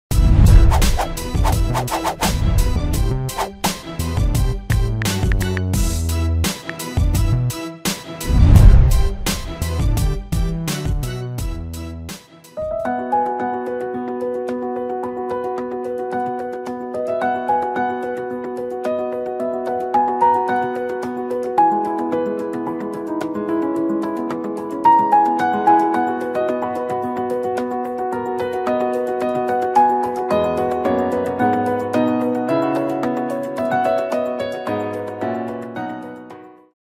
[APPRADIO.PRO] Recurso de Site Integrado para sua Rádio.
A plataforma de desenvolvimento de aplicativos para Rádios - APPRADIO.PRO possui +100 recursos avançados.
Conheça o melhor aplicativo para Rádio.

Já pensou em tornar a sua rádio ainda mais incrível? Agora temos o recurso "Site Integrado" para inovar a sua rádio! Adicione sites diversos em um botão personalizado na tela da sua rádio.